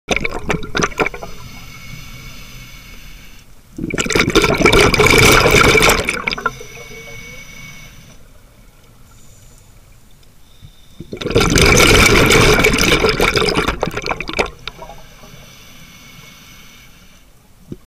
Liquid Image 320: barracuda
Brief clip of barracudas taped through bridge window of Wreck C-53 in Cozumel